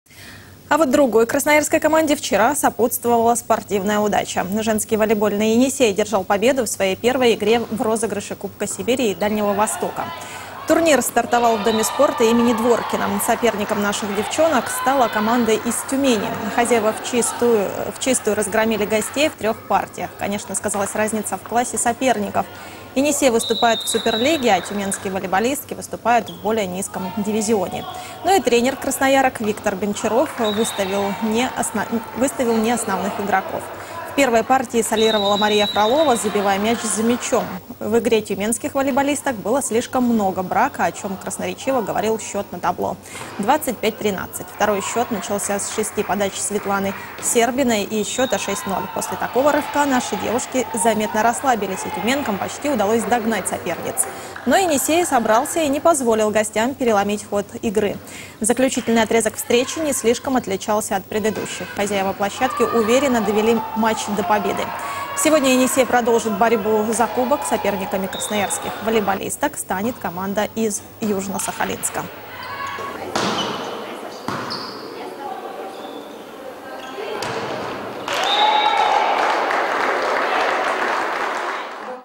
Женский ВК Енисей одержал победу
Женский волейбольный "Енисей" одержал победу в своей первой игре в розыгрыше Кубка Сибири и Дальнего Востока. Турнир стартовал в Доме спорта имени Дворкина. Соперником наших девчонок стала команда из Тюмени.